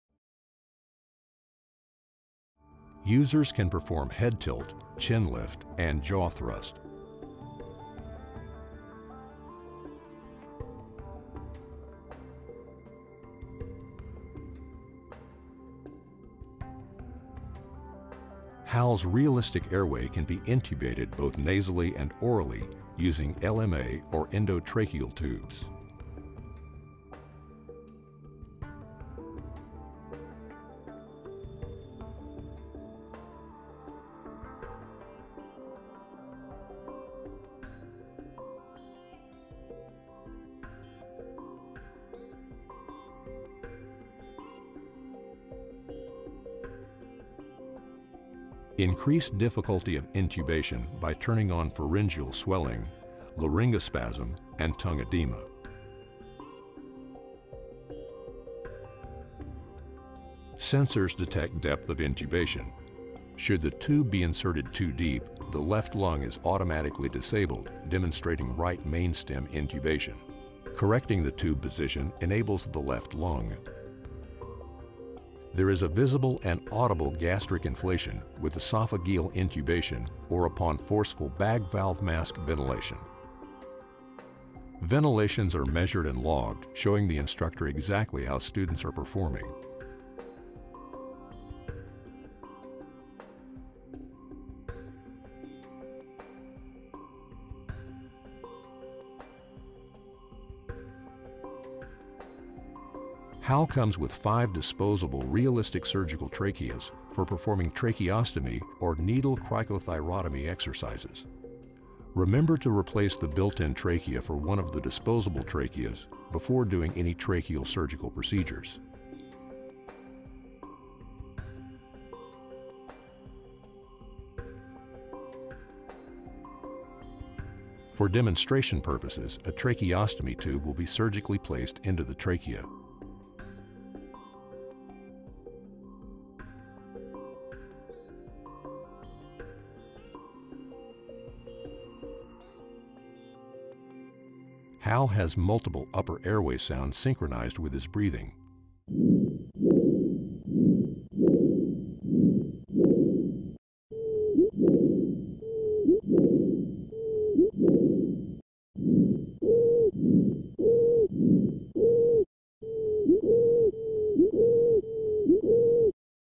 HAL® S3201 - Part 3 - Airway
Hal® 3201 sets a global standard for medical simulation. Tetherless technology allows the communications, compressor, and power supply to be inside HAL®, eliminating external tubes, wires, and compressors. HAL® operates continuously during transport and training can occur in the working environment.

Rush HAL® from the accident scene to the ER, to the ICU, while care providers diagnose and treat his condition using real monitoring and resuscitation equipment. Control HAL® at distances up to 300 meters and between rooms and floors of conventional buildings. HAL® smoothly transitions between physiologic states in response to commands from a wireless PC.